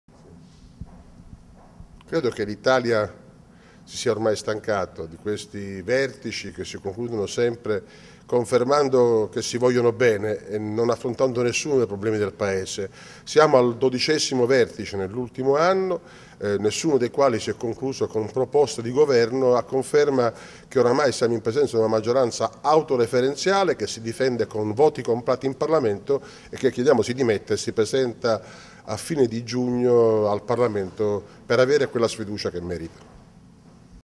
08-06-11 ORLANDO IDV SU SITUAZIONE POLITICA.mpg
Italia stanca dei vertici della maggioranza che non portano a nessuna soluzione per il paese